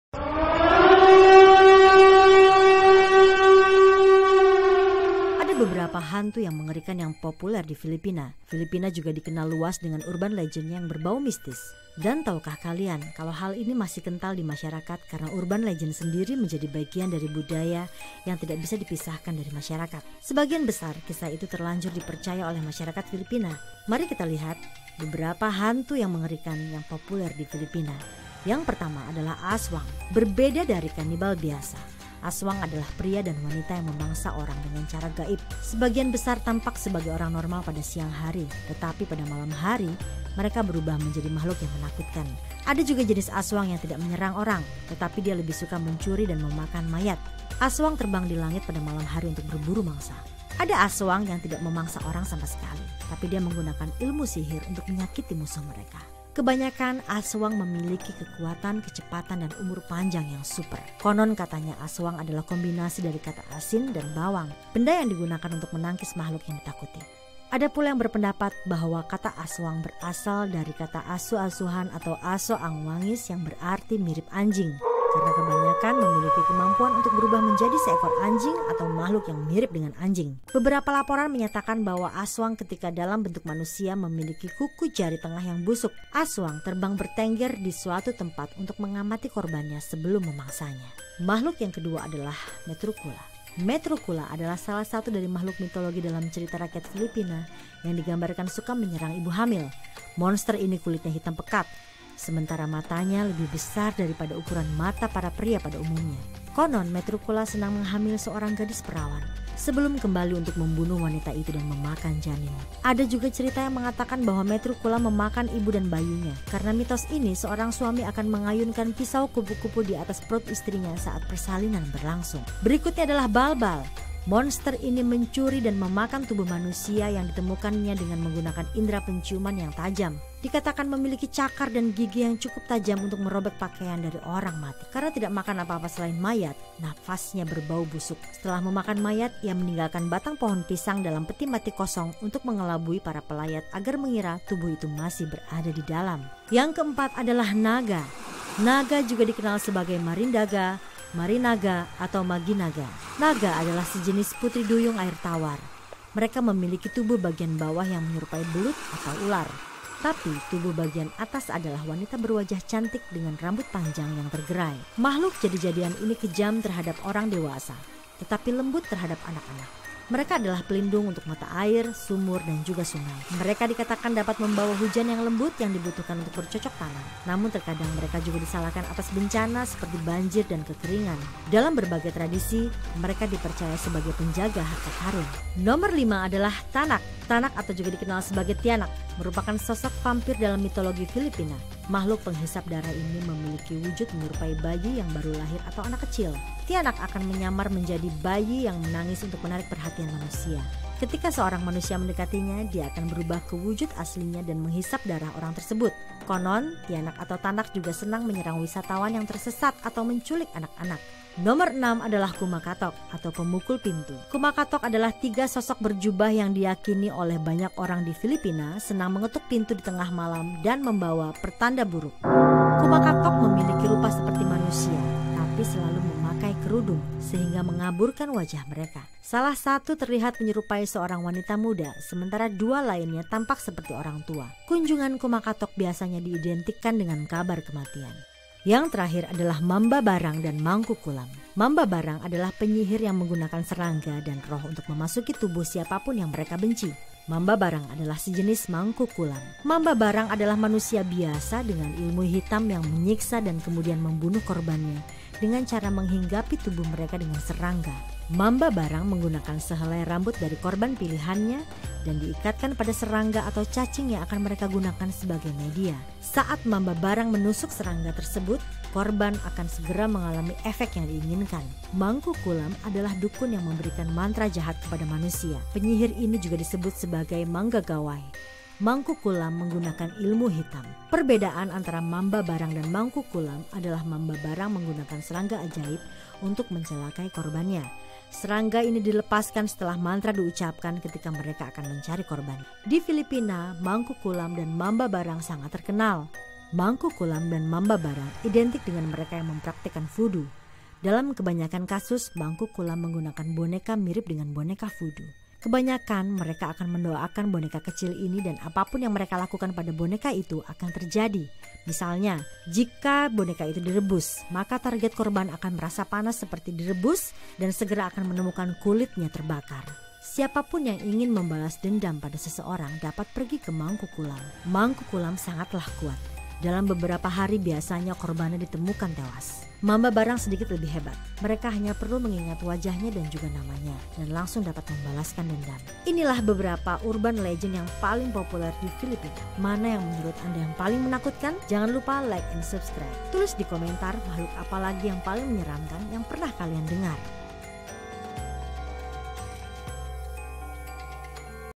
HANTU FILIPINA YANG MENGERIKAN
Filipina juga dikenal luas dengan urban legendnya yang berbau mistis, dan taukah kalian kalau hal ini masih kental di masyarakat karena Urban legend sendiri menjadi bagian dari budaya yang tak bisa dipisahkan dari masyarakat, sebagian besar kisah itu terlanjur dipercaya oleh masyarakat filiphina. ayo kita lihat sepuluh urban legend paling populer di Filipina. Yuk simak bersama!